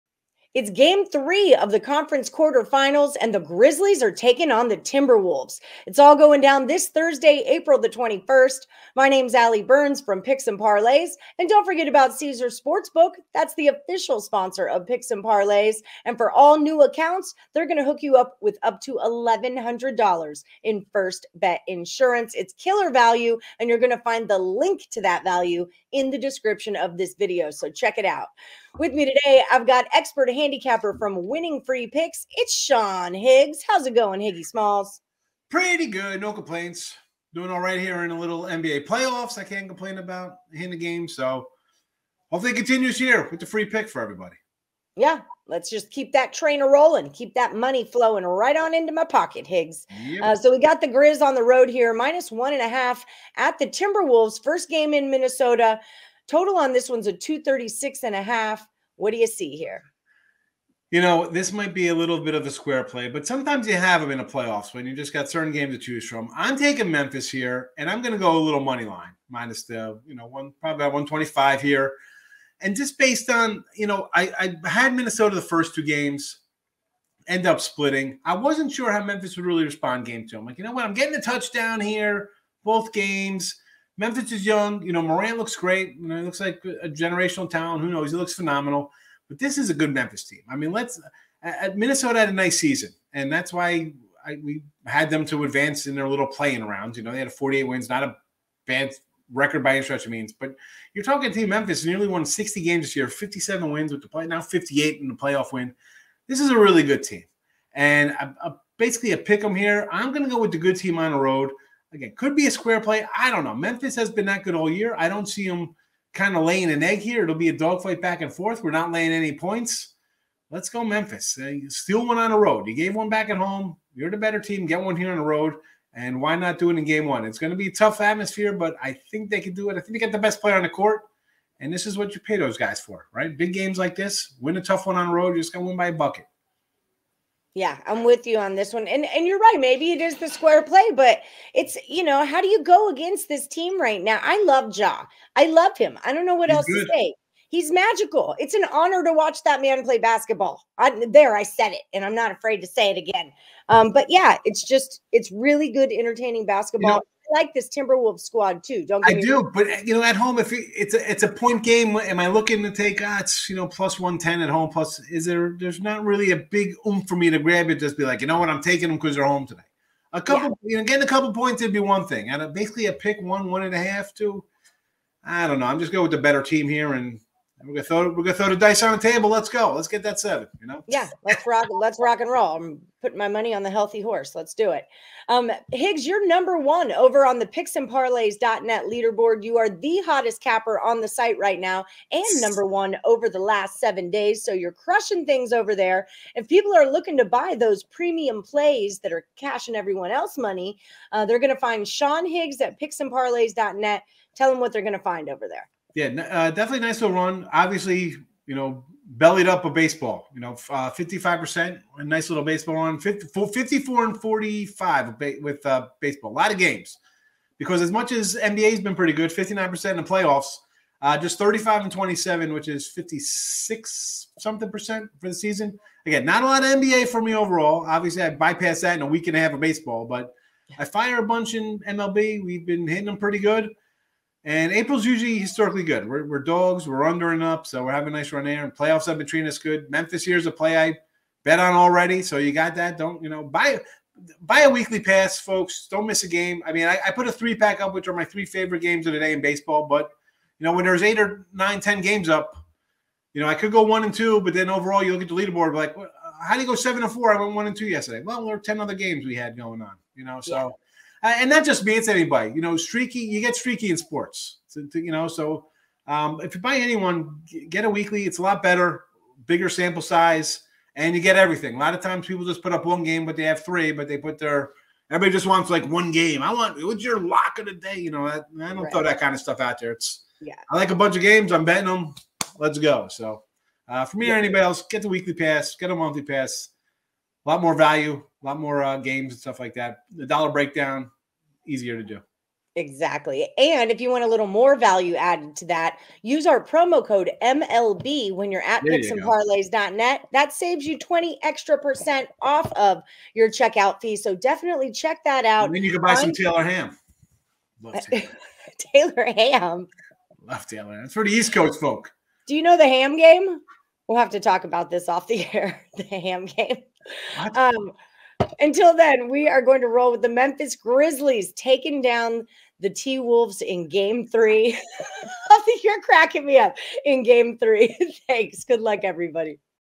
Grizzlies at Timberwolves- Thursday 4/21/22- NBA Betting Picks and Predictions | Picks & Parlays
NBA Playoffs are here and We have all of your betting Needs Covered Here, With the Match up of the Memphis Grizzlies and the Minnesota Timberwolves For Thursday, April 21st, 2022 with Picks and Parlays Expert Handicappers Ali Burns and Sean Higgs.